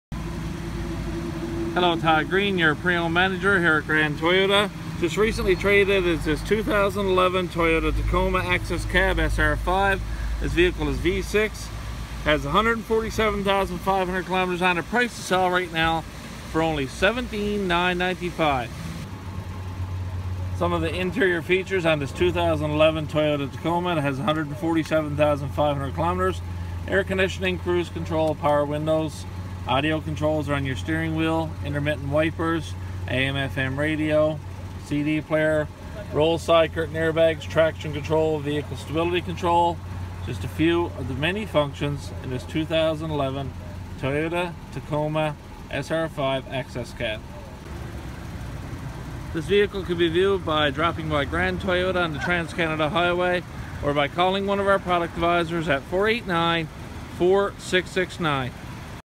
2011 Toyota Tacoma 4WD Access Cab V6 Auto | Grand Toyota
This is a 2011 Toyota Tacoma 4WD Access Cab V6 Auto with 5-Speed A/T transmission Blue[08P1,Speedway Blue] color and Grey interior color.  This video is recorded and uploaded by cDemo Mobile Inspector